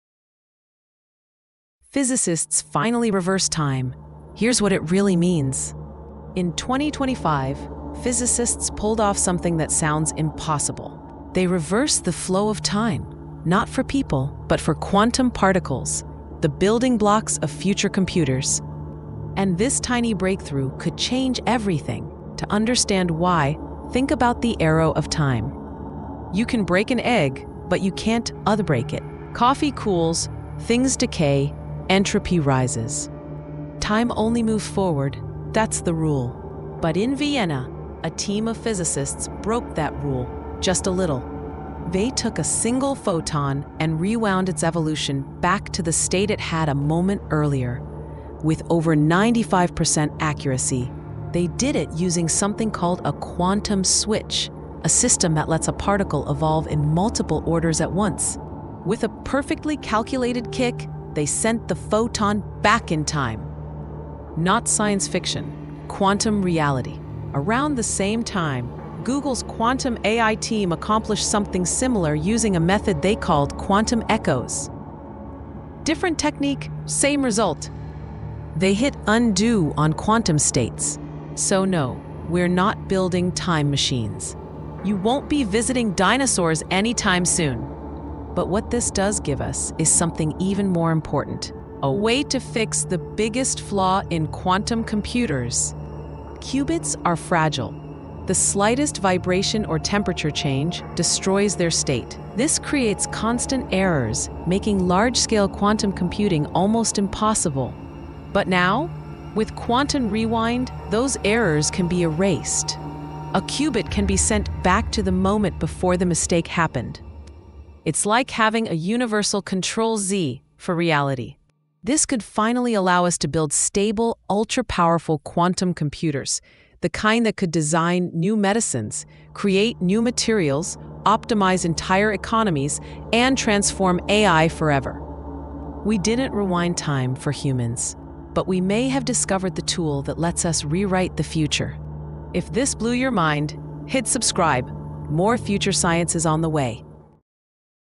Physicists Finally Reversed Time — And It Changes EVERYTHING
Physicists have just pulled off something once thought impossible: they reversed the flow of time for a quantum particle.
Not science fiction — real physics, achieved in 2025.

Using a breakthrough technique called the quantum rewind protocol, scientists restored a particle to a previous state with 95% accuracy.
It’s not a time machine for humans — but it may be the key to fixing the biggest problem stopping quantum computers from becoming reality.

In this video, we break down:

How physicists actually reversed time

What the experiment proves about the quantum world

Why this discovery could unlock error-free quantum computing

How this could lead to huge advances in medicine, AI, materials, and energy

The future isn’t slowing down — it’s rewinding.
✨ Topics we cover:
AI • Space • Quantum • Bioengineering • Fusion • Future Cities • Energy • Robotics

In 2025, physicists achieved the seemingly impossible: they reversed the flow of time for quantum particles. This groundbreaking development in "quantum physics" could dramatically impact future computers, offering a new perspective on the "time paradox". This video offers a simple "science explained" perspective on why this discovery matters, simplifying complex "physics" concepts like the arrow of time, and exploring the implications for "quantum mechanics" and potential "time travel" theories.